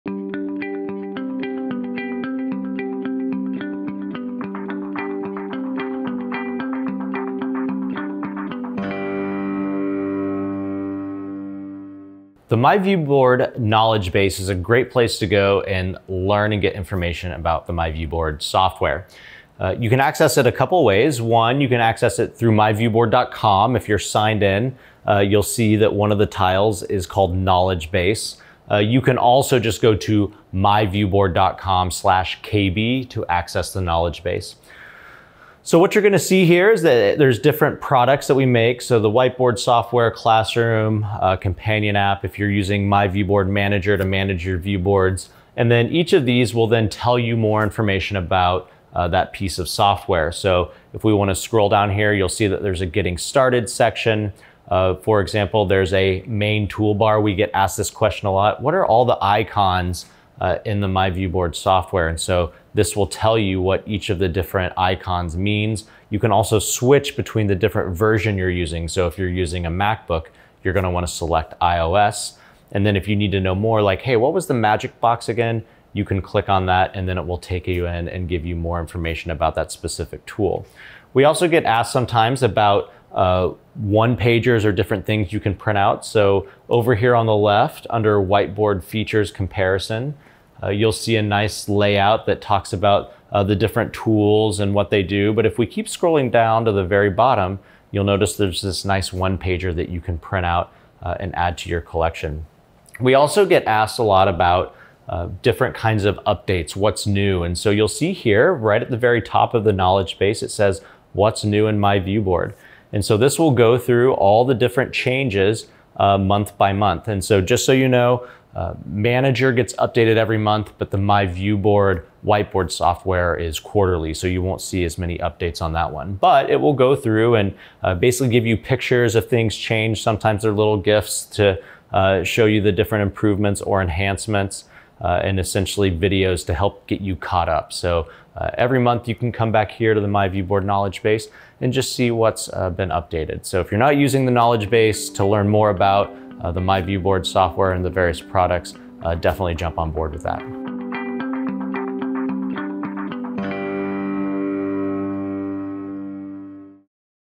ViewSonic Knowledge Base
If you ever have a question about myViewBoard, or need to quickly troubleshoot an issue, just click and search through our easy-to-navigate knowledge base.